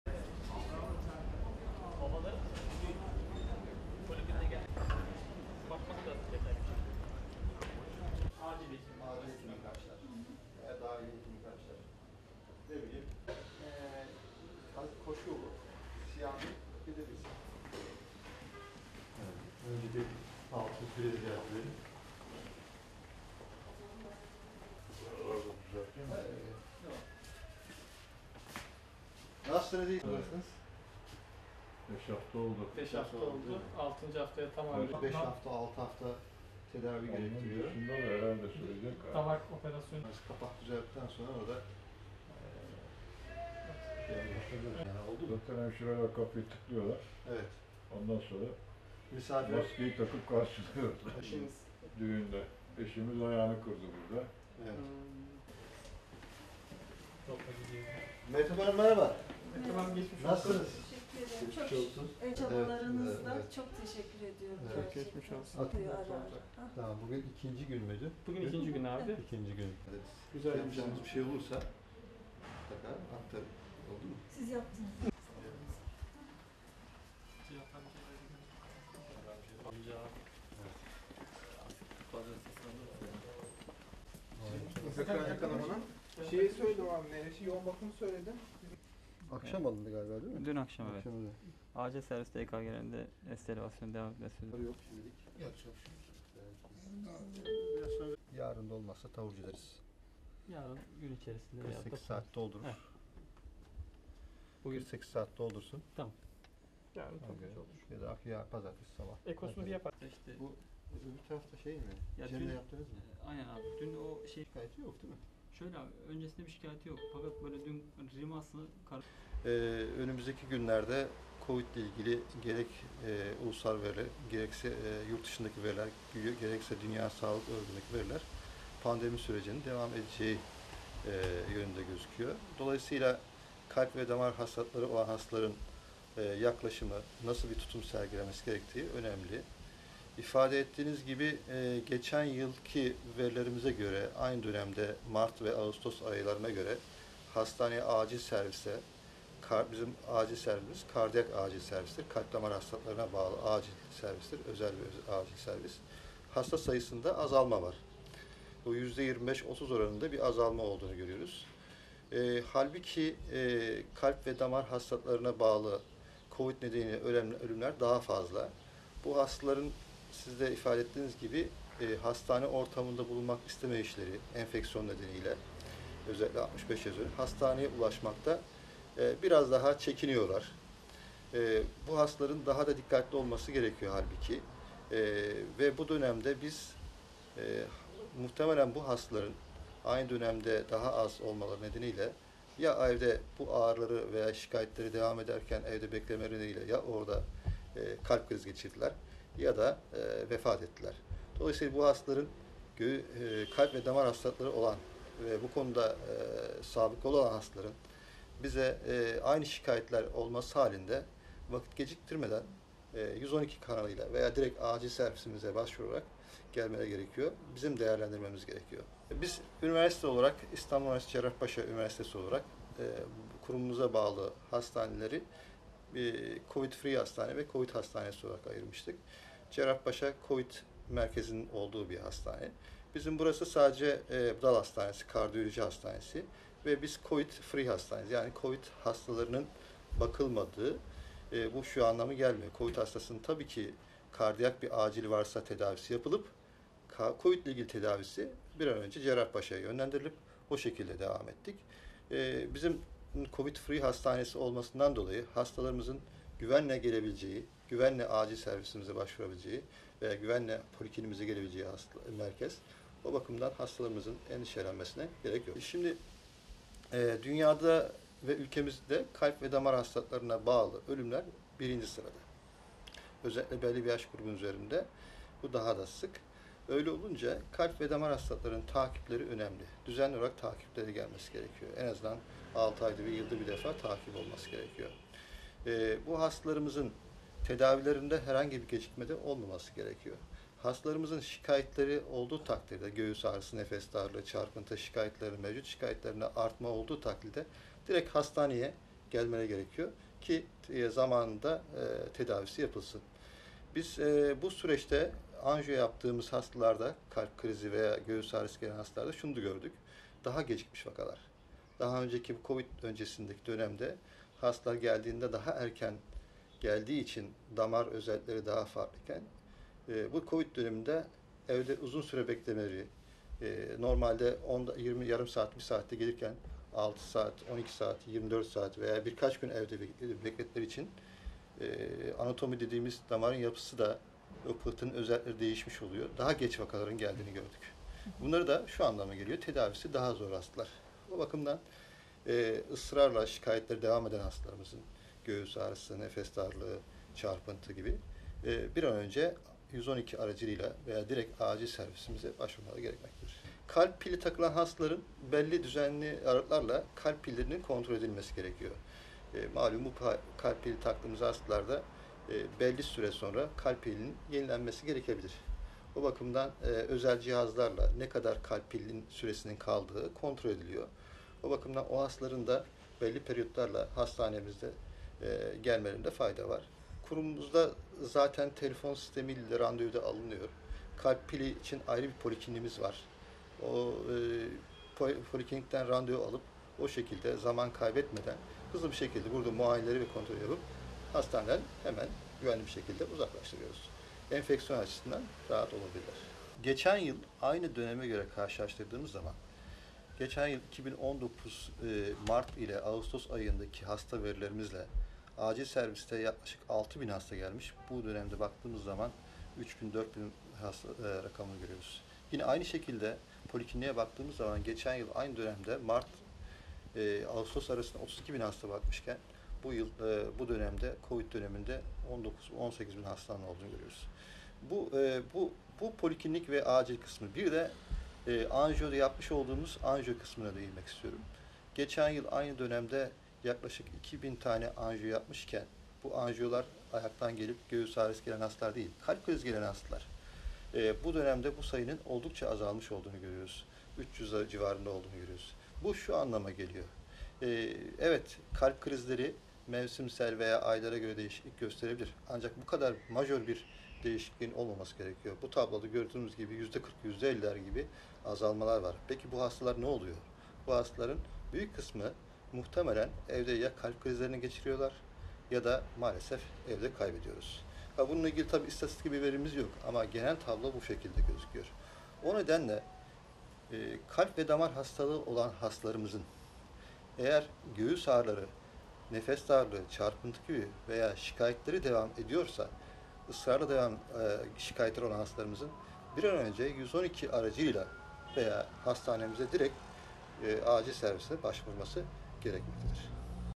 Koronavirüs korkusu kalp hastalarını tedirgin etti: Kalp hastalarına önemli uyarı
İstanbul Üniversitesi Cerrahpaşa-Kardiyoloji Enstitüsü Kardiyolojik Temel Bilimler Ana Bilim Dalı Başkanı Prof. Dr. Ahmet Yıldız, “Geçen yıl aynı döneme göre karşılaştırdığımız zaman,  2019 Mart-Ağustos aylarındaki hasta verilerimize göre acil servise yaklaşık 6 bin hasta gelmiş, ancak bu dönemde baktığımız zaman 3-4 bin rakamını görüyoruz." dedi.

Prof. Dr. Yıldız; “Bu hastaların daha da dikkatli olması gerekiyor. Muhtemelen bu hastaların evde ağrıları ya da şikayetleri devam ederken,  kalp krizi geçirdiklerini ya da vefat ettiklerini düşünüyoruz. Dolayısıyla kalp ve damar hastalıkları kişilerin, şikayetleri olması halinde hastaneye başvurmaları gerekiyor.” diye konuştu.

Dünyada  ve Türkiye’de kalp ve damar hastalıklarına bağlı ölümler birinci sırada. Kalp ve Damar hastalıklarına sahip kişilerin, düzenli olarak muayene olması ve takip edilmesi hayati önem taşıyor. Ancak koronavirüs salgını nedeniyle kalp damar hastası pek çok kişi, virüse yakalanma korkusuyla ciddi şikayetleri dahi olsa hastanelere başvurmaktan çekiniyor.

İstanbul Üniversitesi Cerrahpaşa-Kardiyoloji Enstitüsü Kardiyolojik Temel Bilimler Ana Bilim Dalı Başkanı Prof. Dr.  Ahmet Yıldız,  geçtiğimiz yılın aynı dönemine göre kardiyoloji bölümüne başvuruların oldukça azaldığını ve muhtemelen kişilerin evde kalp krizi geçirdiğini ya da vefat ettiğini dile getirerek, kalp hastalarının kovid-free yani kovid hastalarının bakılmadığı hastanelere çekinmeden başvurabileceğini söyledi.

Bu hastaların kontrol ve tedavilerinin aksatılmadan yapılması gerektiğini ifade eden Yıldız, “Geçen yıl aynı döneme göre karşılaştırdığımız zaman, 2019 Mart-Ağustos aylarındaki hasta verilerimize göre acil servise yaklaşık 6 bin hasta gelmiş ancak bu dönemde baktığımız zaman 3-4 bin rakamını görüyoruz. Polikliniğe baktığımız zaman geçen yıl aynı dönemde Mart-Ağustos arasında 32 bin hasta bakmışken, bu yıl kovid döneminde 18 bin hastanın olduğunu görüyoruz. Bu poliklinik ve acil kısmı, bir de anjiyo kısmına değinmek istiyorum. Geçen yıl aynı dönemde yaklaşık 2 bin anjiyo yapmışken, bu yıl bu sayının 300 civarında olduğunu görüyoruz” dedi.